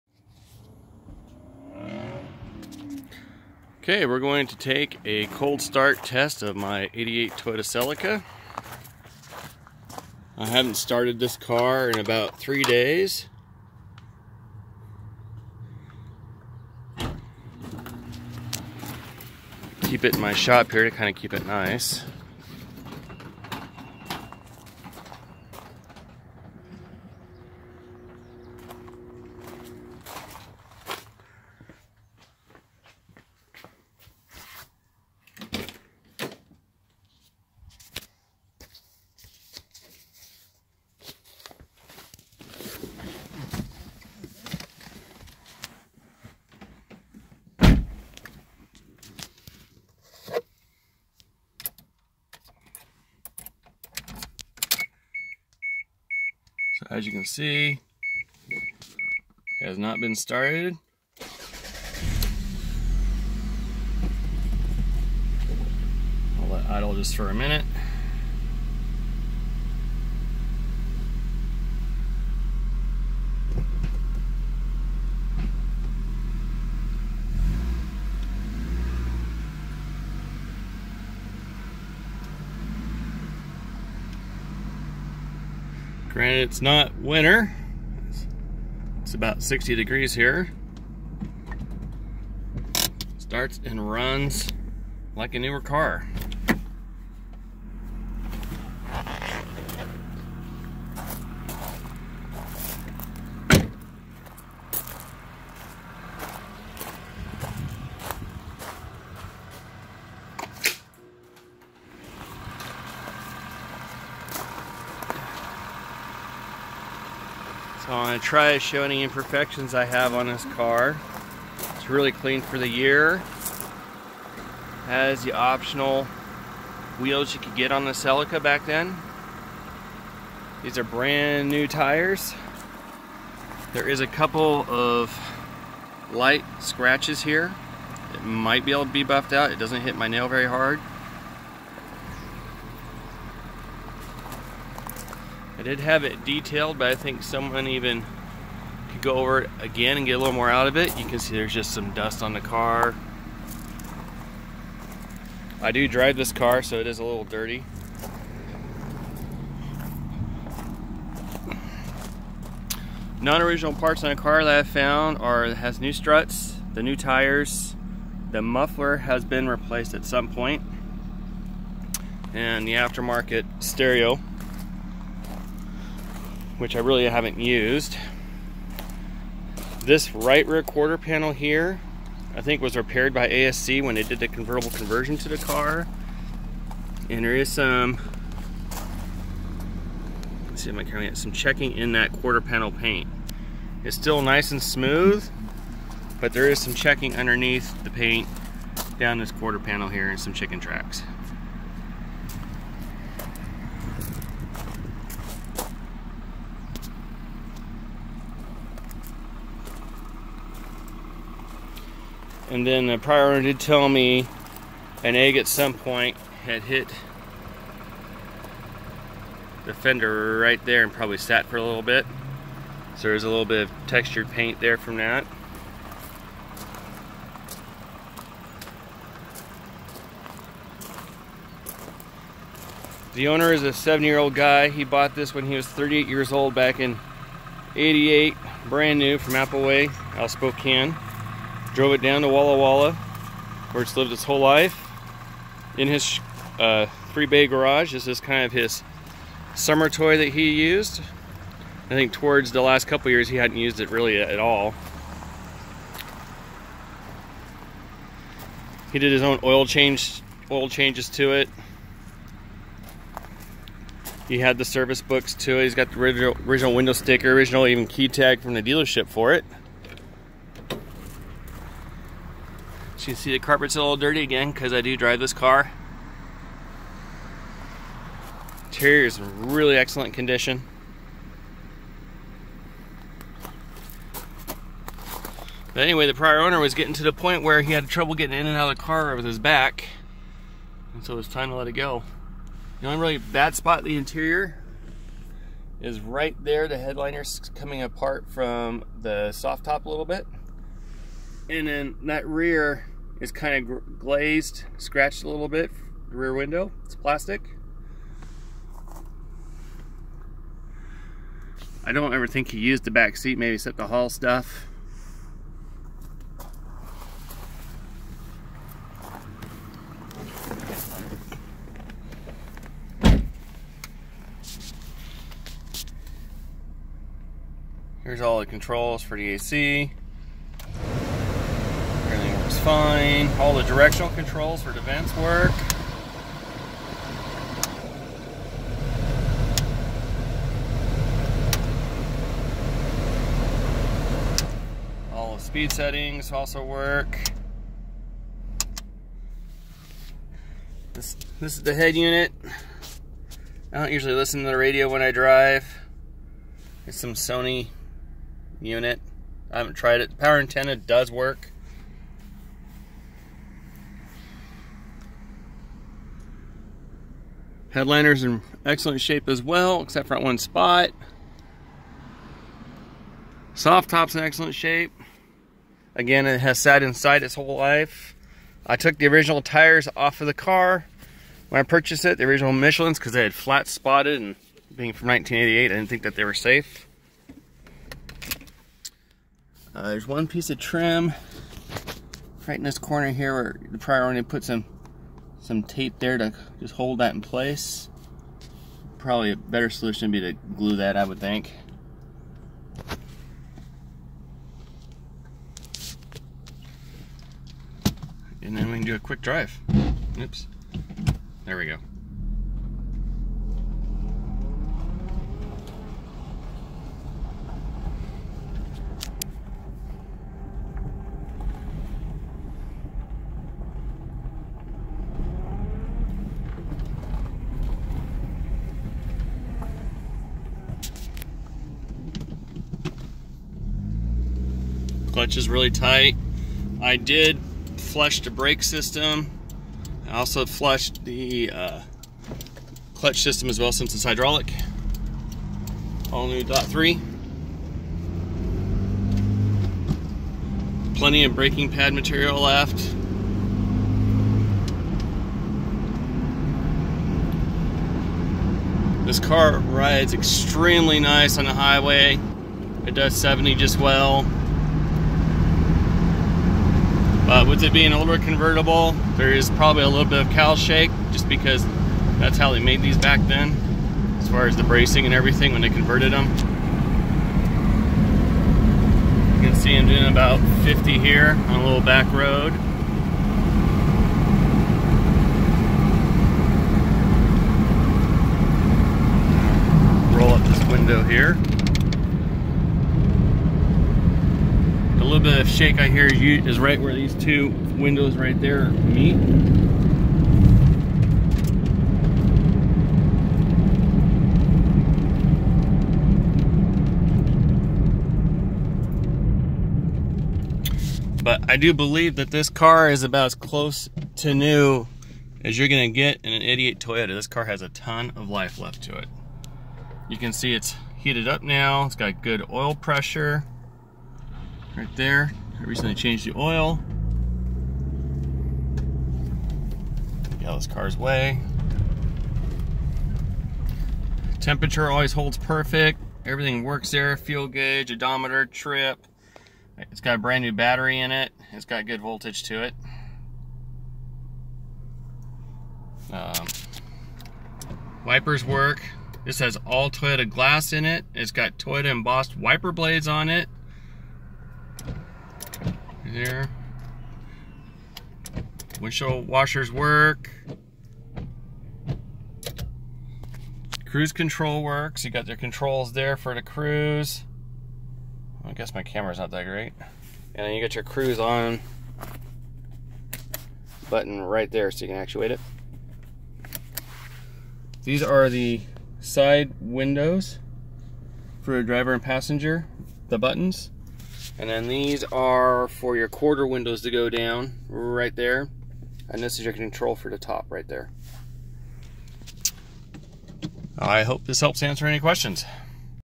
1988 Toyota Celica Cold Start/Walk Around/Drive
Cold starting Celica after sitting for 3 days, walk around, interior walk and drive.